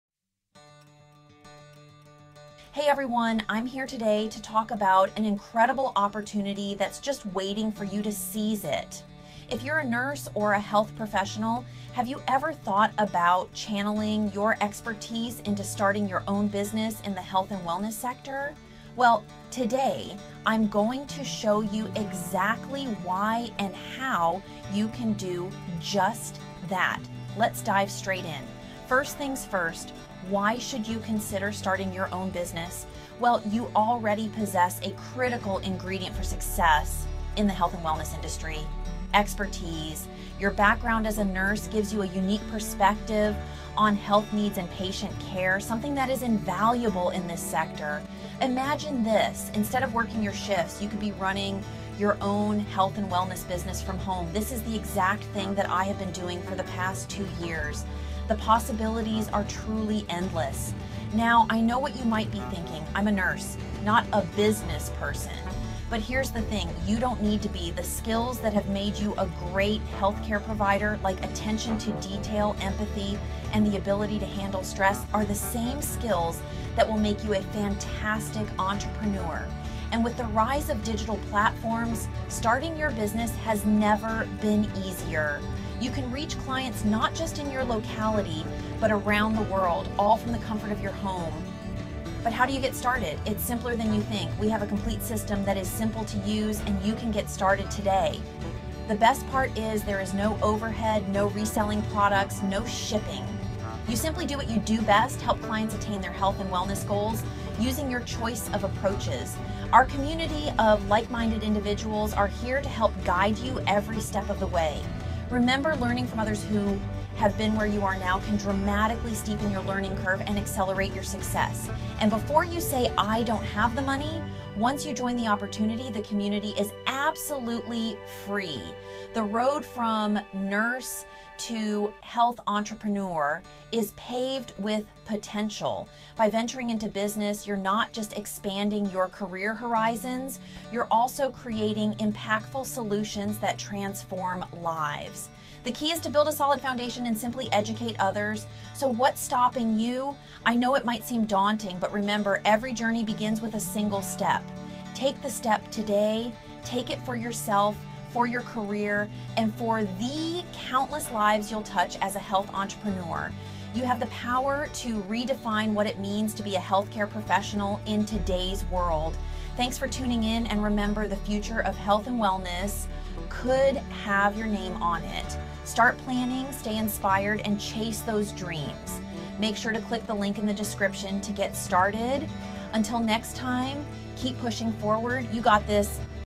Business opportunity for Nurses and health care professionals
Are you a healthcare professional yearning to break free from the confines of the traditional healthcare system? Or just looking for extra income? Look no further! In this video, we unveil the secrets to launching your very own home-based health business, leveraging the power of nature's remedies and innovative wellness products.

Don't miss out on this empowering opportunity to turn your passion for health into a profitable business venture. Watch now and take the first step towards building the life of your dreams!